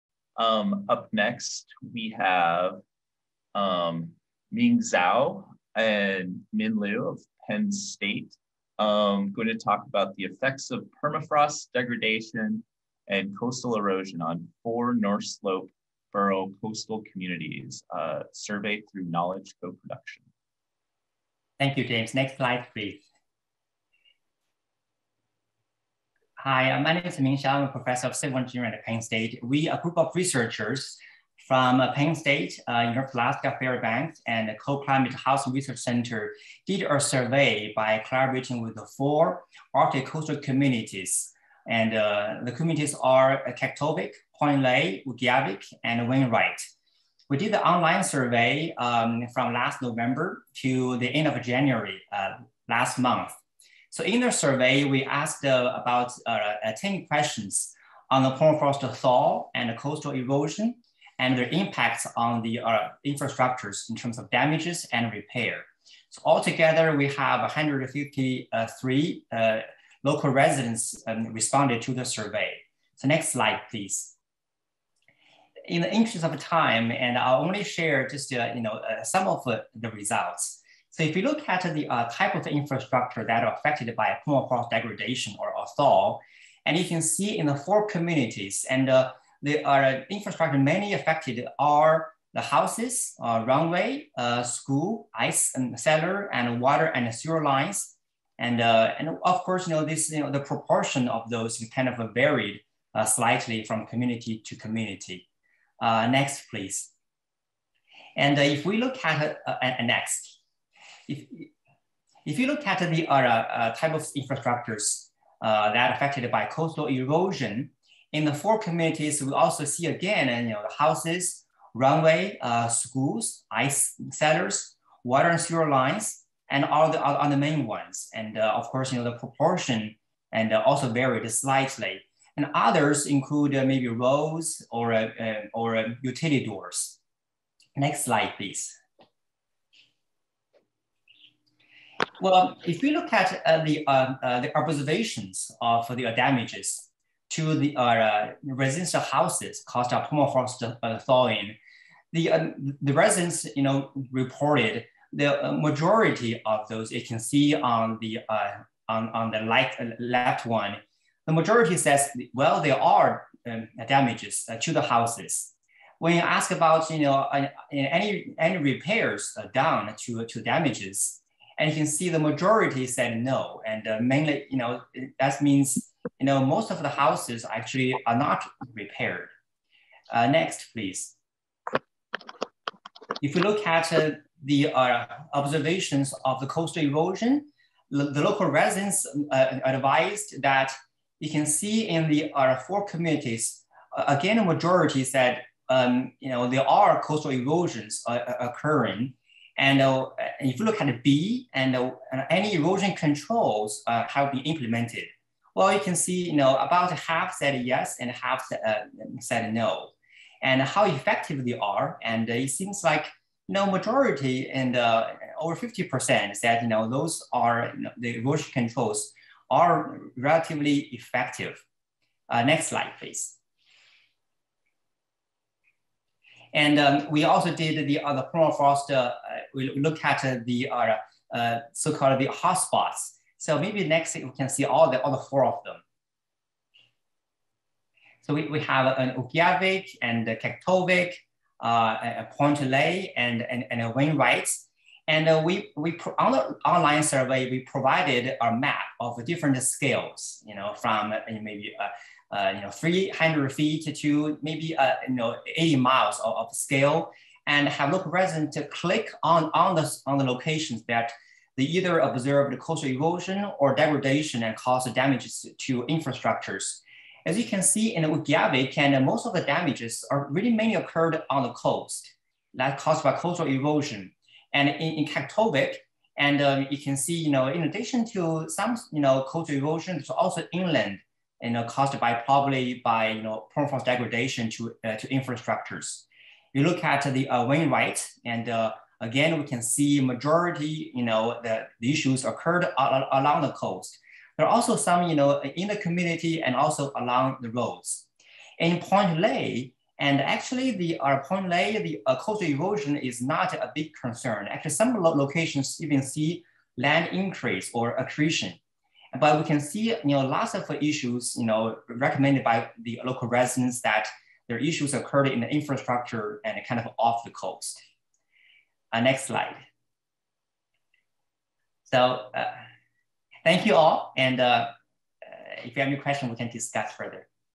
Effects of Permafrost Degradation & Coastal Erosion on 4 Alaska N. Slope Borough Coastal Communities
Presented by Ming Xiao & Min Liew

Effects of permafrost degradation and coastal erosion on four Alaska North Slope Borough coastal communities: Survey through knowledge co-production